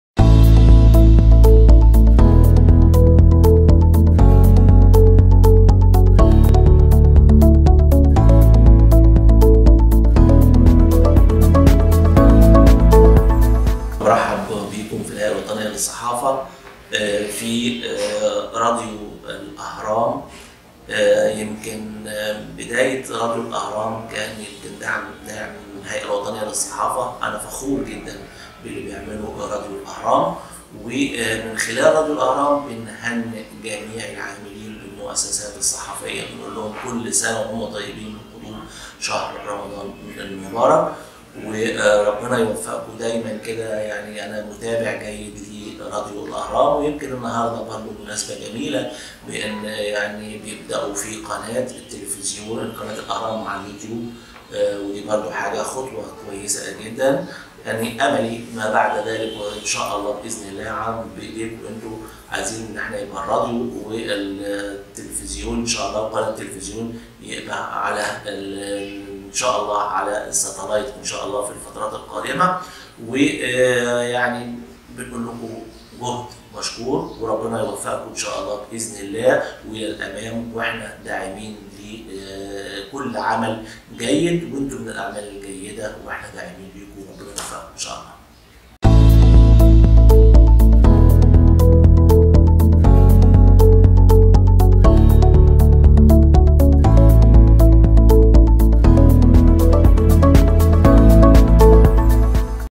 عبد الصادق الشوربجى رئيس الهيئة الوطنية للصحافة الشوربجي يهنئ المؤسسات القومية بحلول رمضان
راديو الأهرام | عبد الصادق الشوربجى رئيس الهيئة الوطنية للصحافة
 الشوربجي يهنئ المصريين و المؤسسات القومية بحلول رمضان الكريم